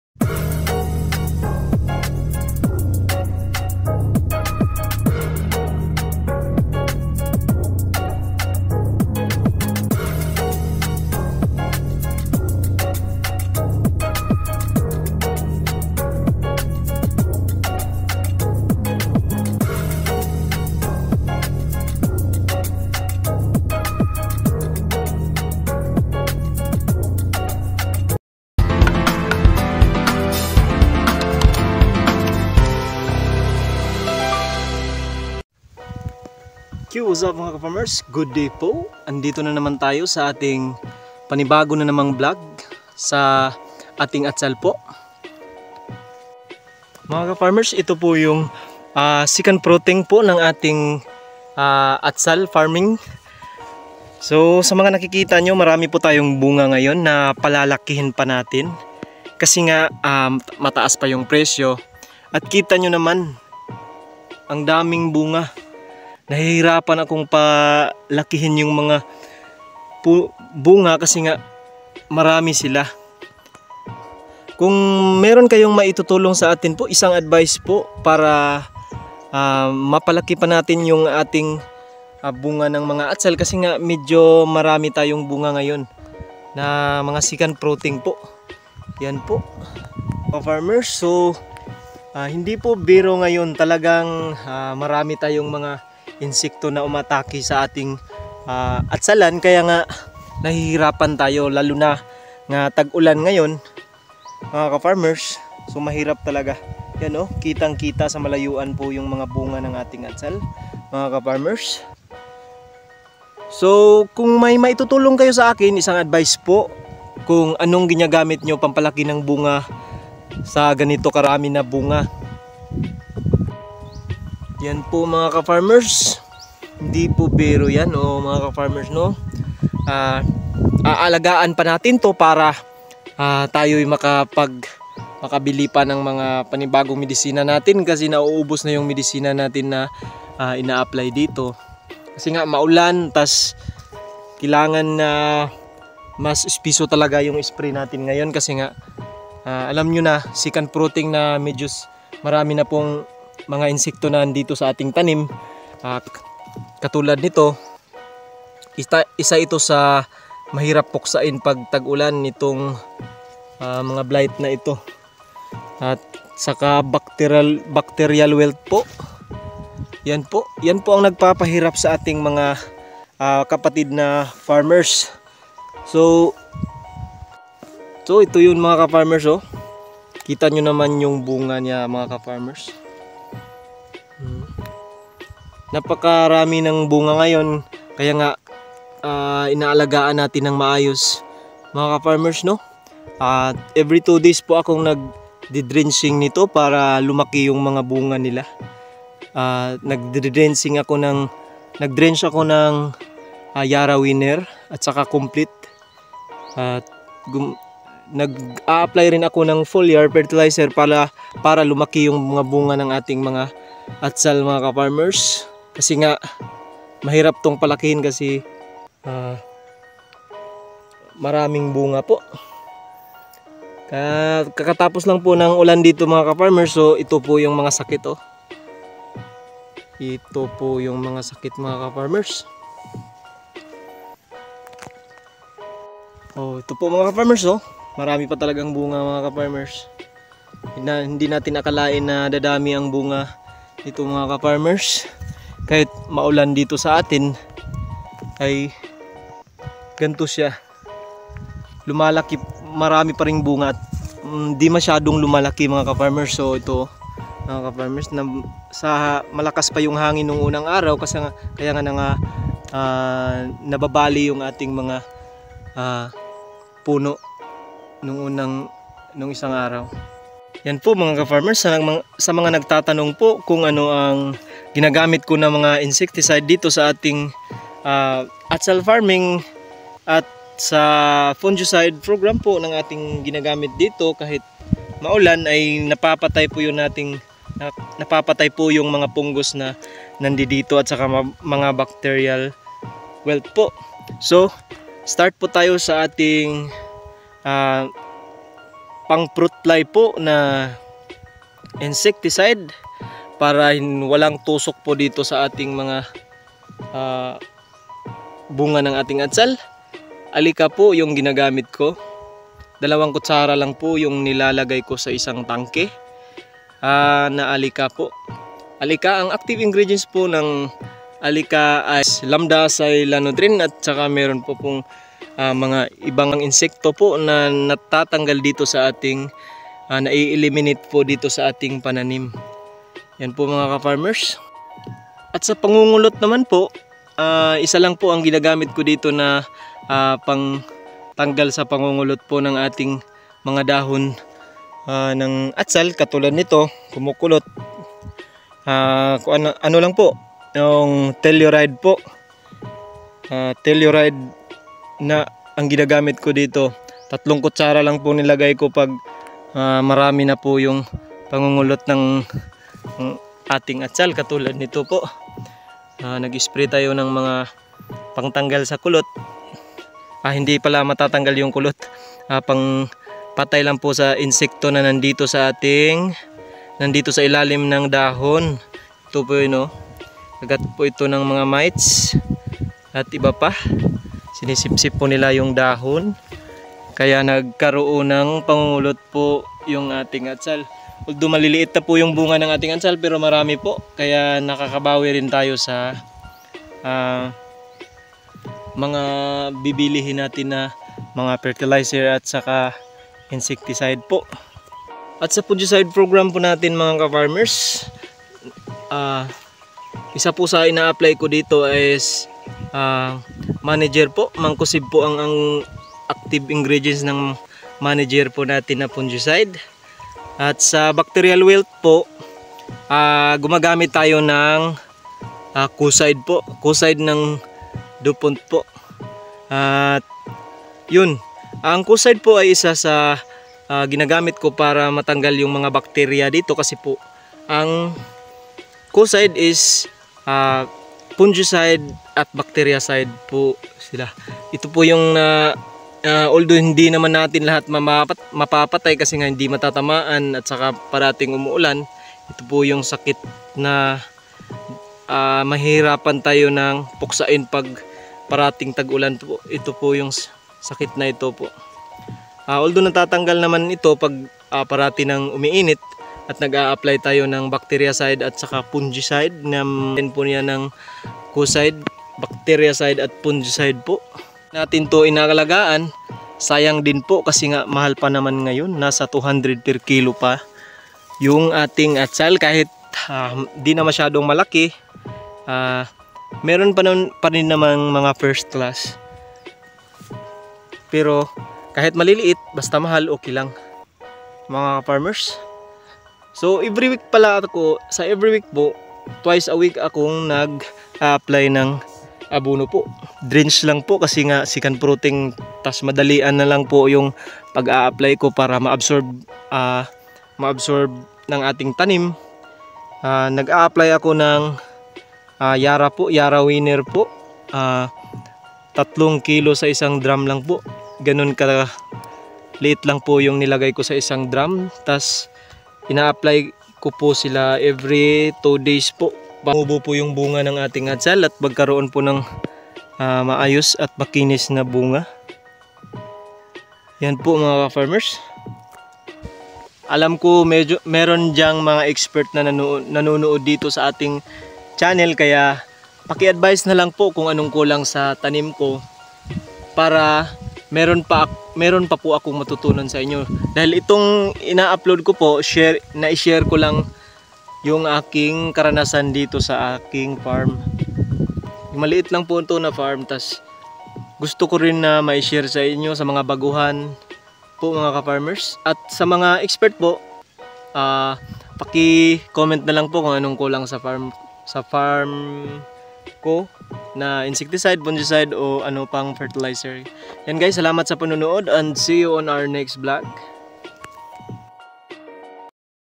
"ATSAL" BELL PEPPER FARMING SECOND FRUITING 280/kilo#bellpeppers
BELL PEPPER "ATSAL" FARMING
Amazing second fruiting of bell pepper or atsal...
Nakachamba sa presyo 200+...
Paano palakihin ang bunga ng second fruiting na atsal...
Insecticide
Fungicide
Fertilizer